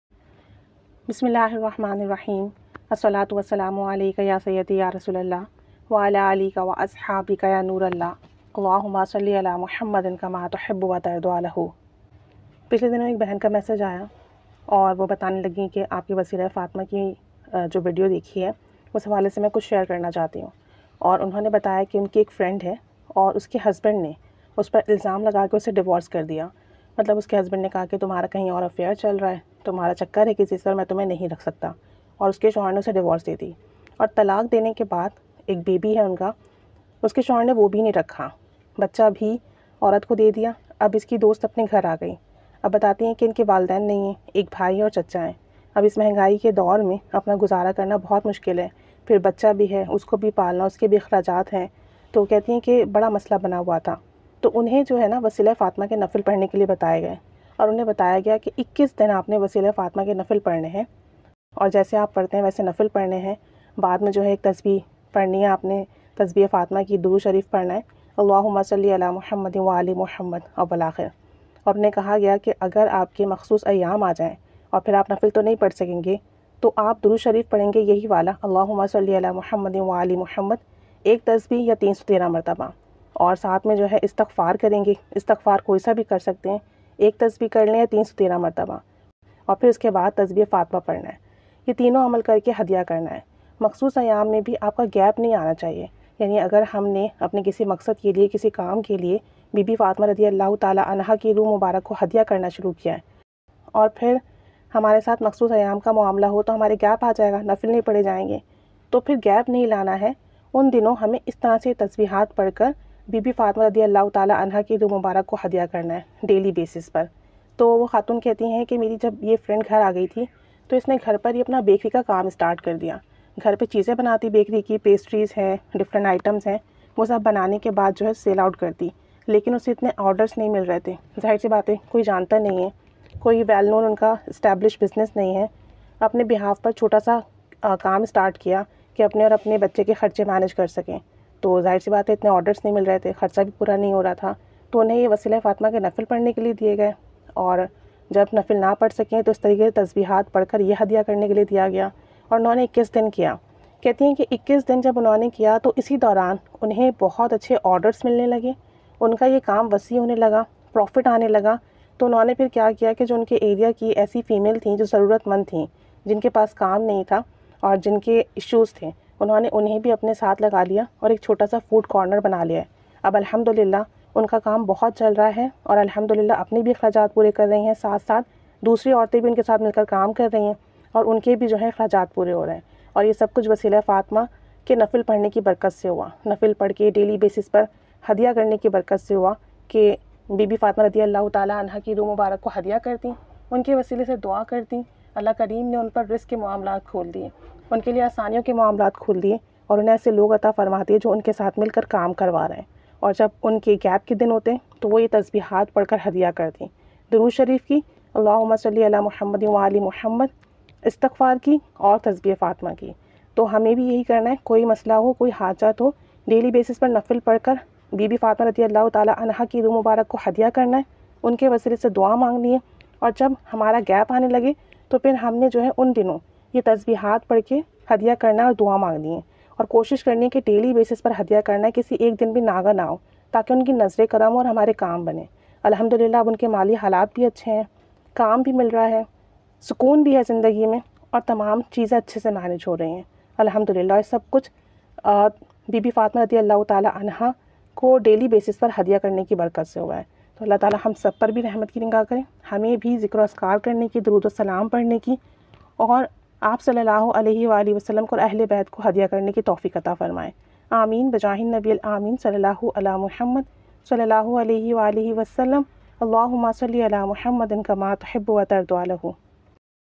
waseela e fatima ke nafil | miracle and motivation | wealth started increases
waseela fatima nafil ka tareeqa
2 rakat nafil parhain ( surah fatiha ke sure ikhlas parhai 21 bar ya jitni bar asai ho), nafil ke bad durood e aal e bait parh kr 11 bar, or tasbehh fatima parh kr sari parhai  ka sawab bibi jan syada fatima radi Allah anha ko hadiya krain or unke wasele se, waseel e fatima ke zariye se dua krain. Jo bhi Allah se mange gay mile ga. Dua qabool hogi. Hajat puri hogi masle hal hongay. Pareshani khatam hogi.

 Namumkin mumkin hoga in Sha Allah, lakho logo ka azmaya hua amal. ek behen ne ye nafil parh kr hadiya kiya dua ki to unka bht bara masla hua 13 saal purana problem hal hua he. alhumdulillah durood e ibrahim parh kr hadiya kiya or karam hua.

new channel link. yahn durood ki video English me hongi in Sha Allah

(apne mushahidat miracle share krain or sadqa e jaria ka hissa bne)

👉Whatsapp durood group

👉Whatsapp astaghfar group